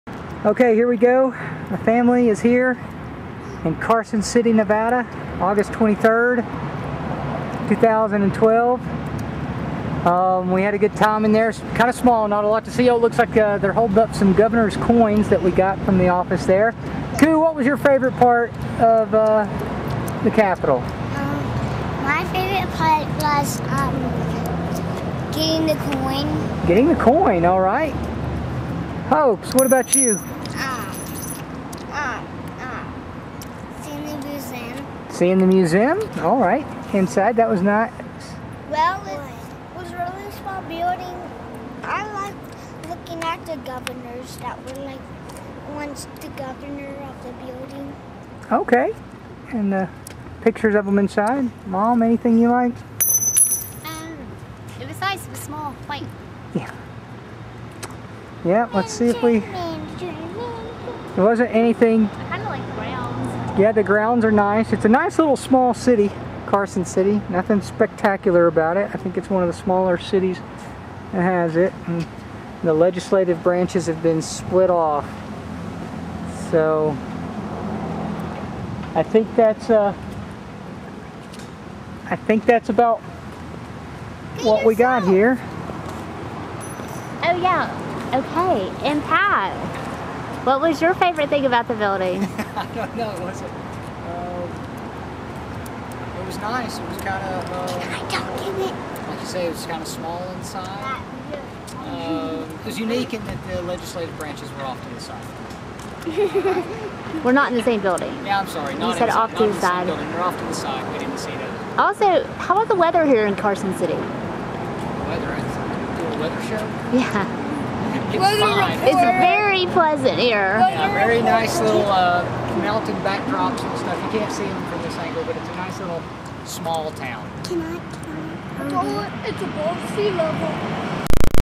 State Capitol of Nevada Visit #2 2012
Our visit to the Nevada state capitol building in Carson City on August 23, 2012